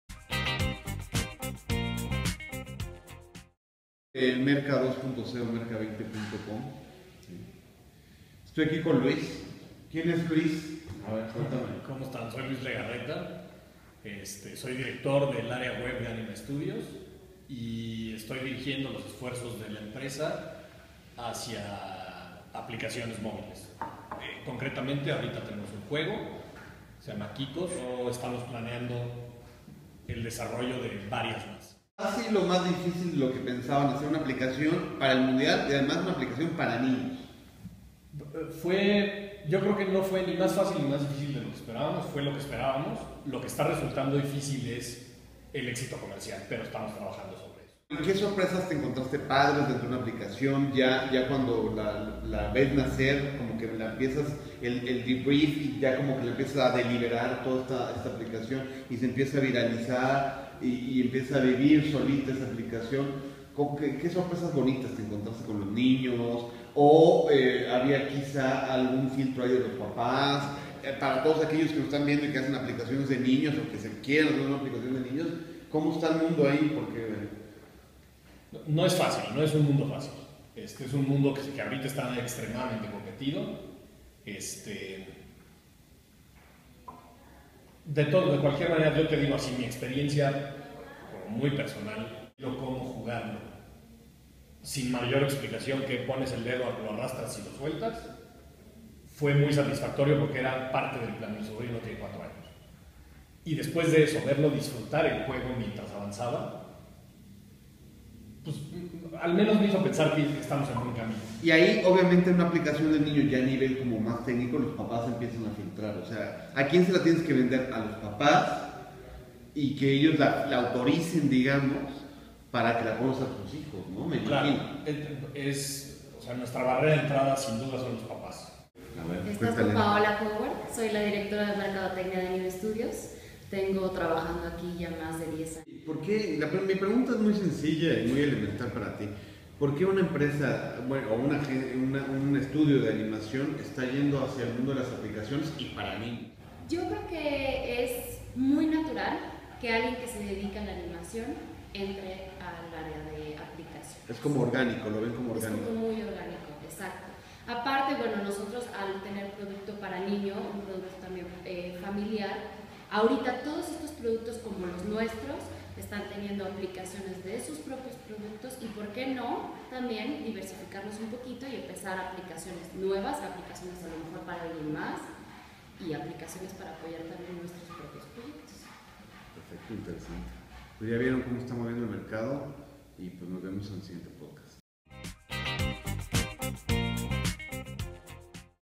Merca 2 0
Entrevista a Luis Legarreta y Paola Howard de Ánima Estudios